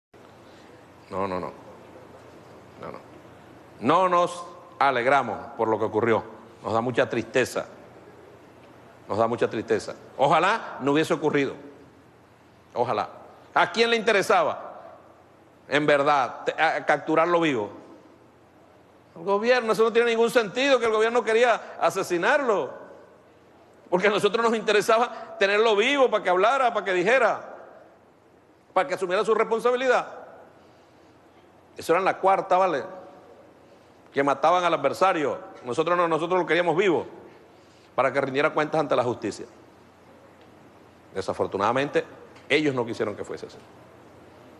En VIDEO| Diosdado Cabello da DATOS INEDITOS sobre Caso de Oscar Pérez!
Leave your comments in the comments section!